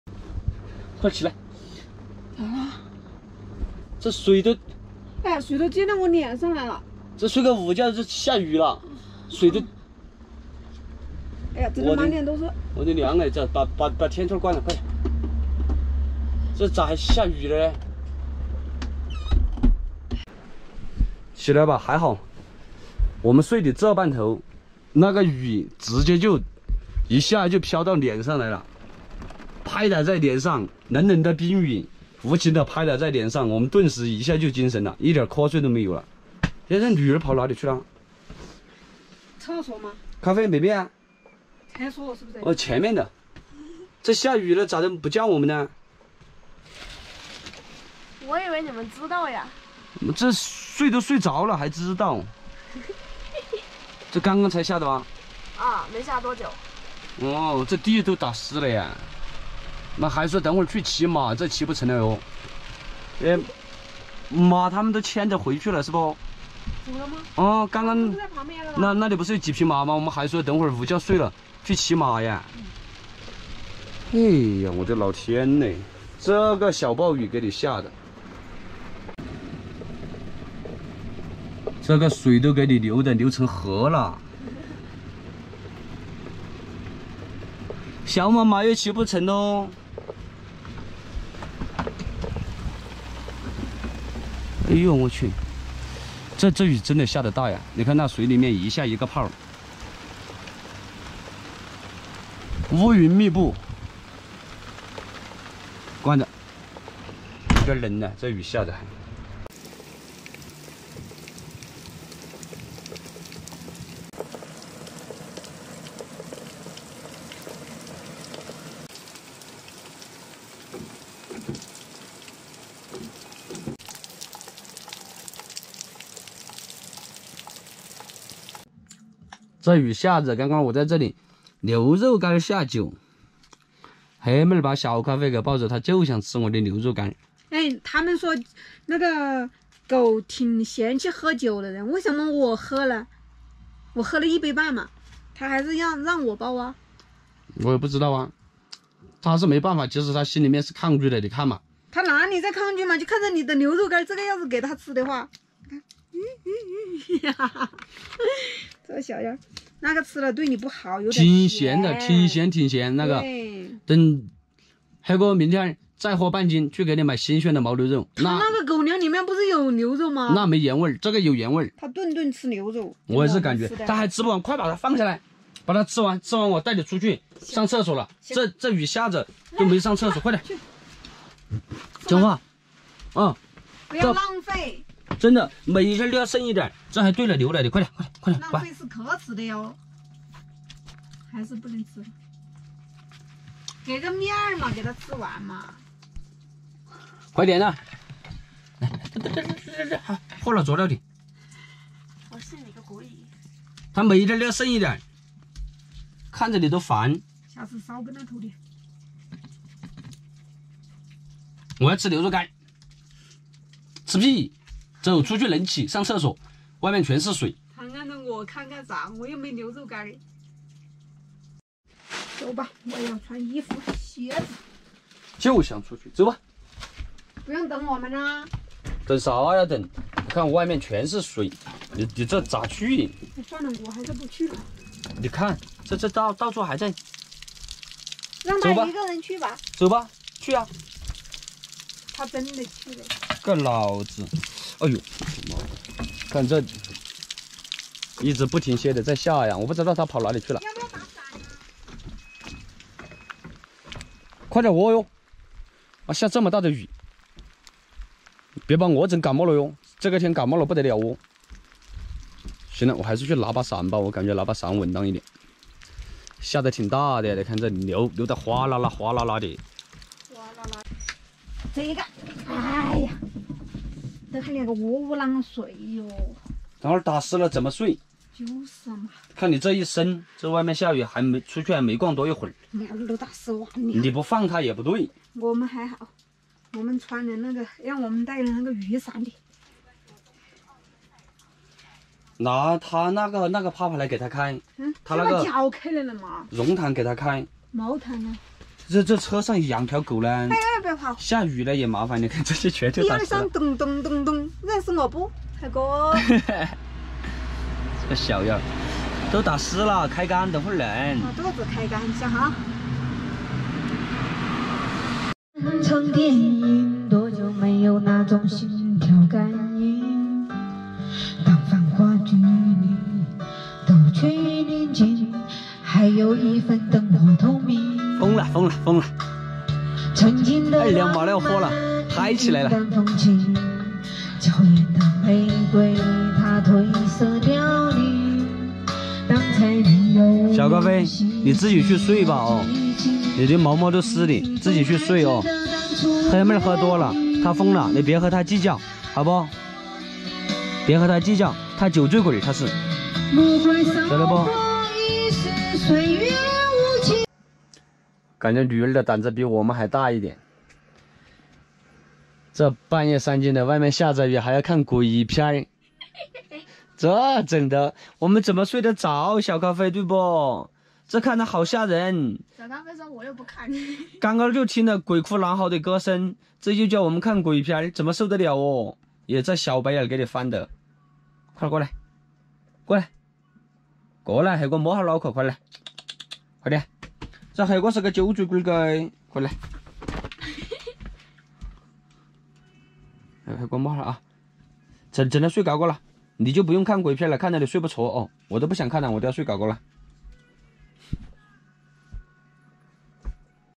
房车紧急预警，车顶漏水，一家三口里睡得正香，突如其来的大暴雨拍打在脸上【868房车旅行】
80后夫妻记录房车生活
每天更新旅行见闻，房车生活
Update travel information every day, RV life
更多精彩视频